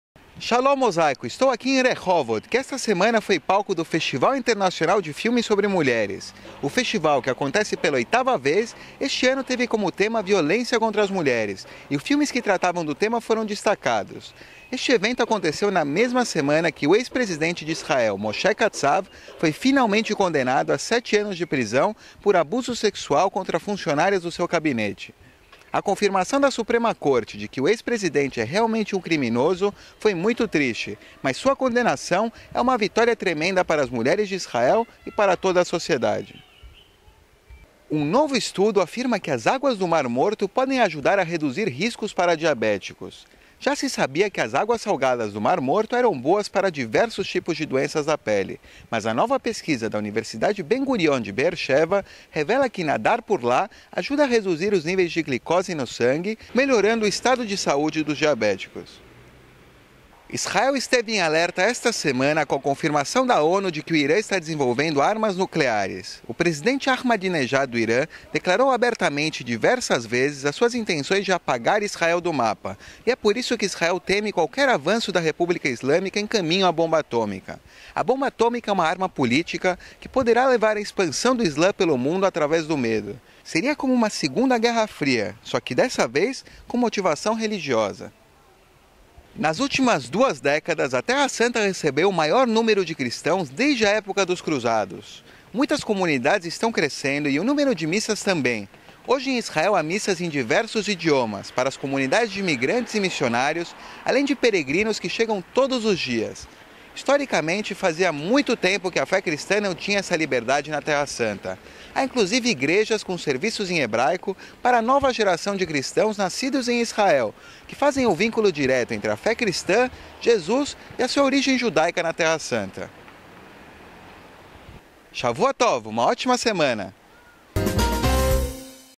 Águas do Mar Morto ajudam diabéticos e mais
Esta semana em Rehovot, com o Festival Internacional de Filmes sobre Mulheres - que teve como tema "Violência contra mulheres". Na semana do festival, o ex-presidente Moshe Katzav, acusado de assédio sexual foi condenado a 7 anos de prisão definitivamente.
Um novo estudo afirma que as águas salgadas do Mar Morto podem ajudar diabéticos.
Alerta em Israel por causa do relatório da ONU que confirma a intenção do Irã de construir armas atômicas.
Boa notícia para a fé cristã em Israel que desfruta de liberdade que não teve na Terra Santa desde a época dos Cruzados, em torno de 500 anos atrás.

Nosso correspondente em Israel, Gabriel Guzovsky, com as últimas notícias lá da terrinha.

Horários:
Domingo às 22h - Canal 13 da NET
Quartas às 21h - Canal 9 da NET e Canais 72 ou 99 da TVA